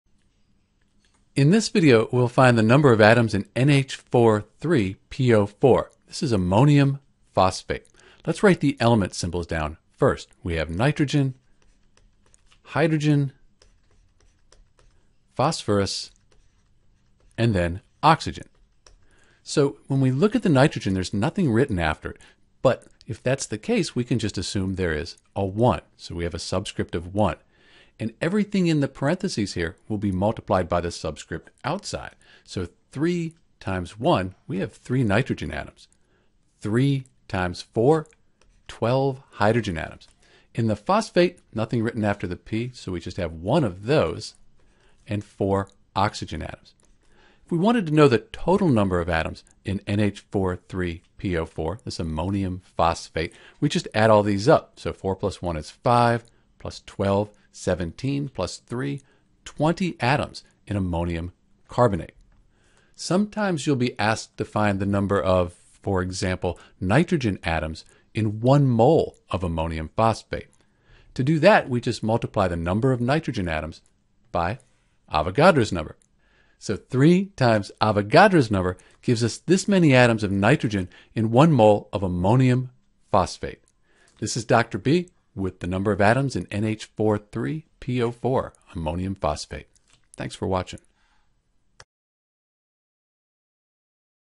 How to Find the Number of Atoms in (NH4)3PO4 (Ammonium phosphate)
To find the total number of atoms in (NH4)3PO4 (Ammonium phosphate) we’ll add up the number of each type of atom. The small number after the element symbol is called the subscript and this tells us how many of that atom are in the compound. For example, H2 means there are two Hydrogen atoms.

Finding the number of atoms in Ammonium phosphate